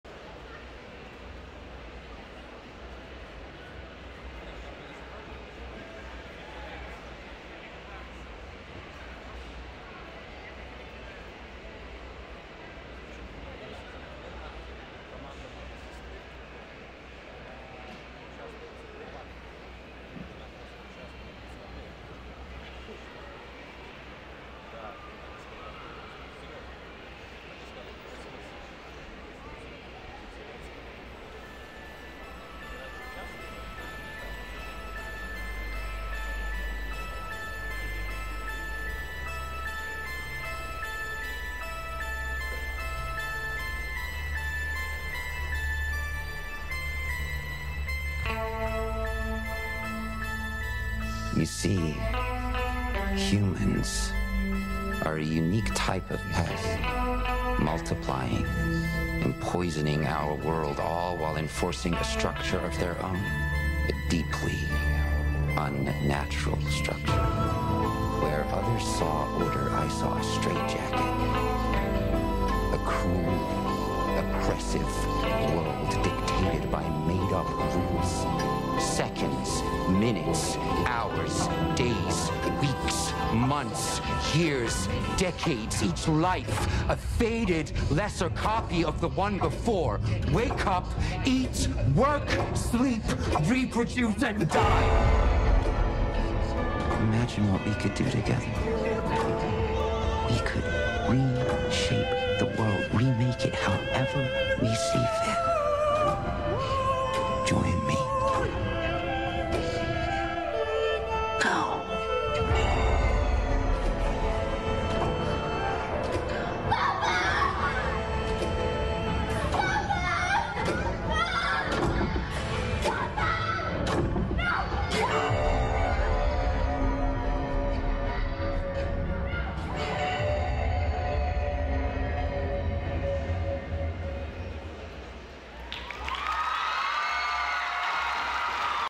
Test subject 011: Vecna and Eleven from Stranger Things
WinterCon VII Riga, Latvia 14 January 2023
Cosplay contest Day 1 Block A

Fandom: Stranger Things
Characters: Henry Creel (Vecna), Eleven
Performance title: Test subject 011
Cosplayers/Cosband: Alex (@alexcodezion), Halcynda (@halcynda)